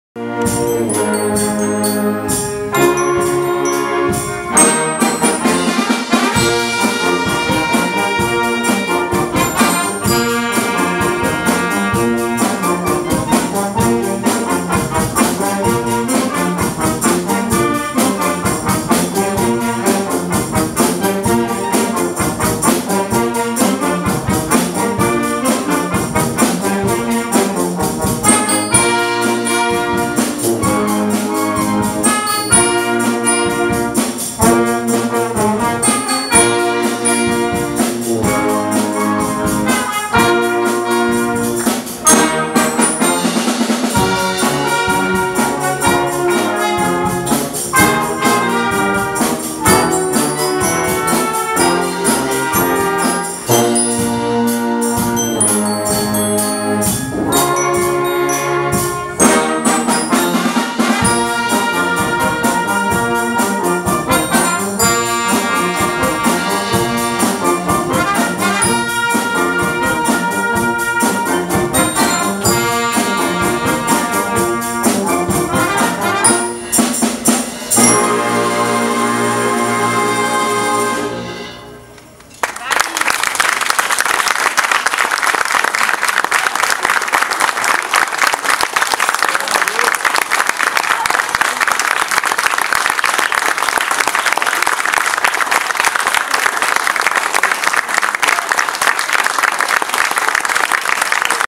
Giovaninbanda 2017 Inaugurazione museo
Campus estivo musicale Giobaninbanda 6, a Druogno dall'1 all'8 luglio 2017, organizzato da Anbima VCO. Direttore Massimiliano Pidò